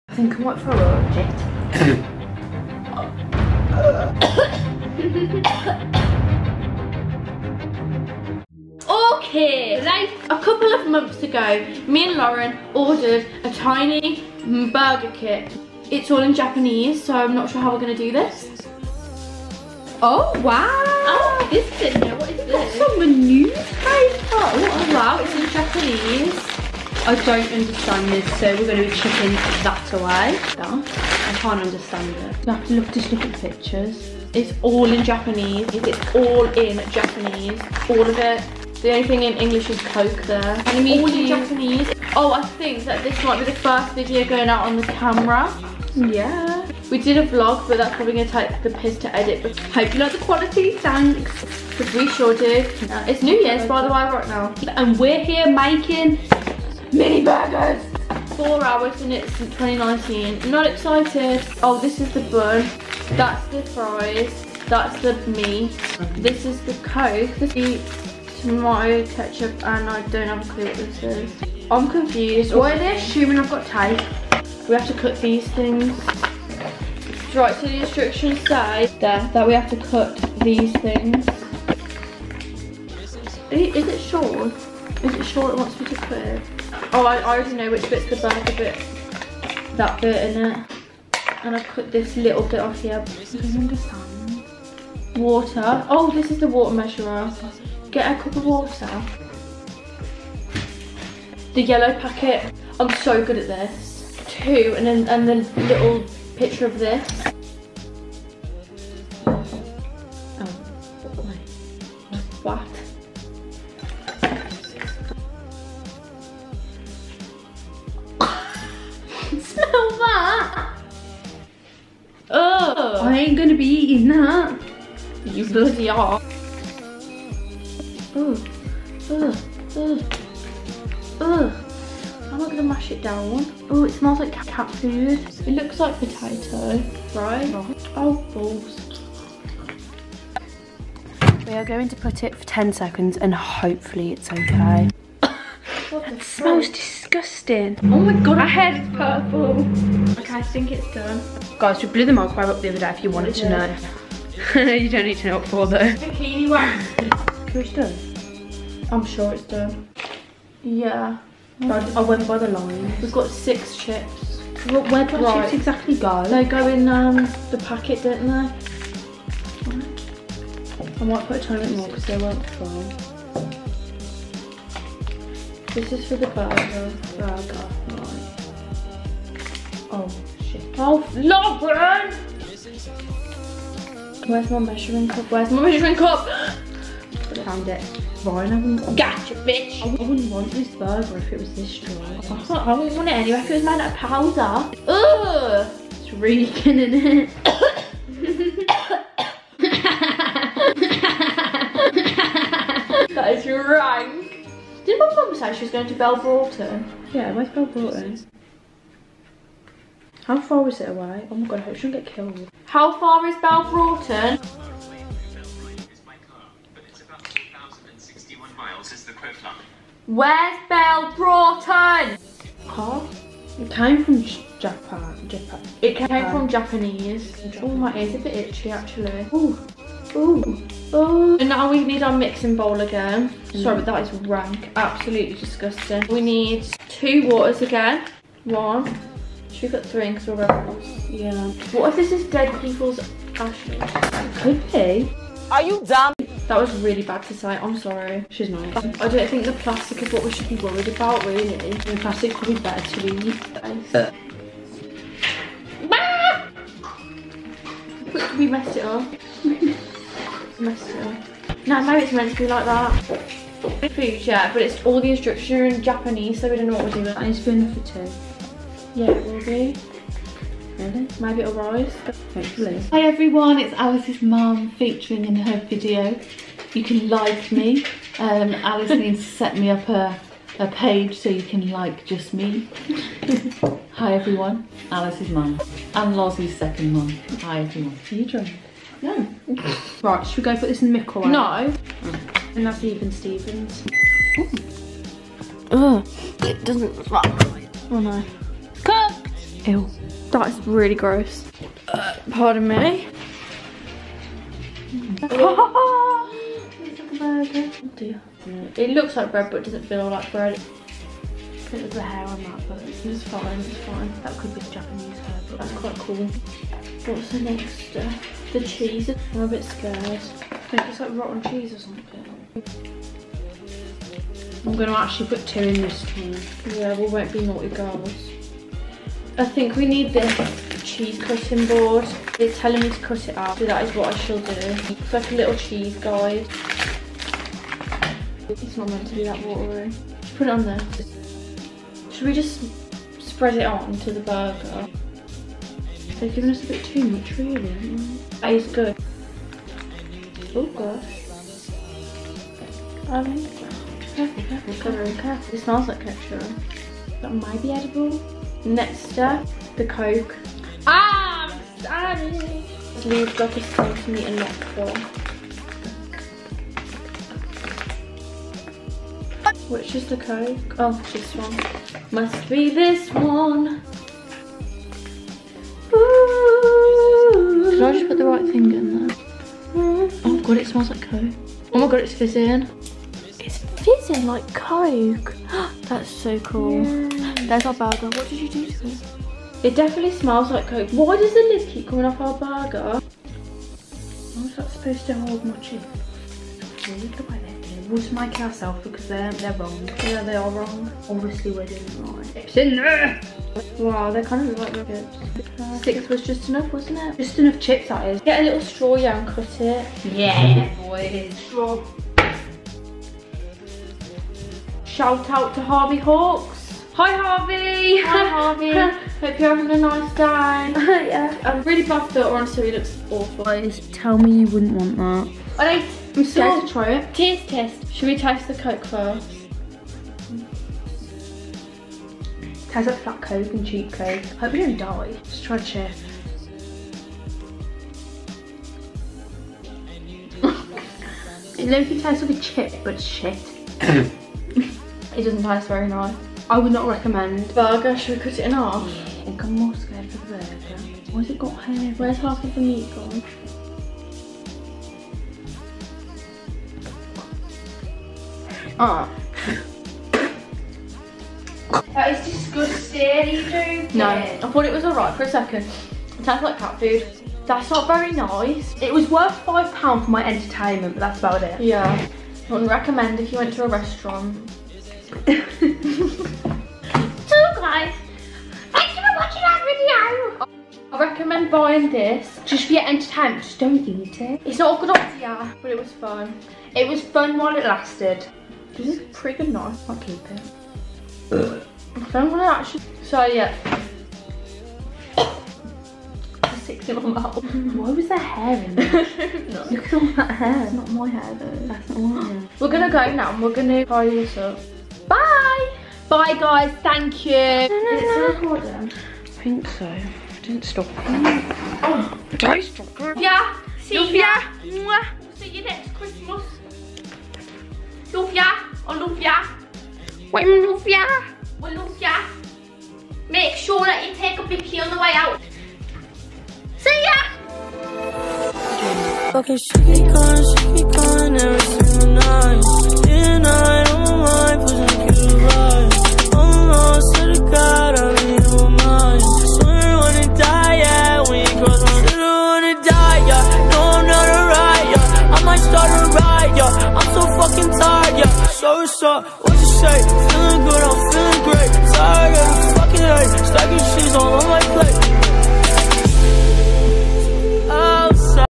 makin tiny ass mcdonalds
we nearly got food poisoning from this vid, so please like :)
THANKYOU EVERYONE FOR WATCHING!!